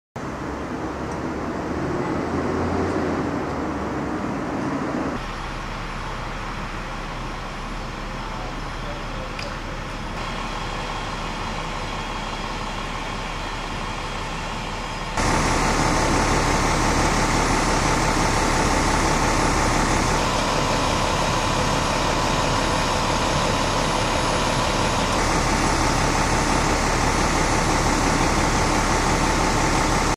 1323 Vine St Stabbing
Video from the scene of a stabbing that occurred at 1323 Vine St in Over the Rhine.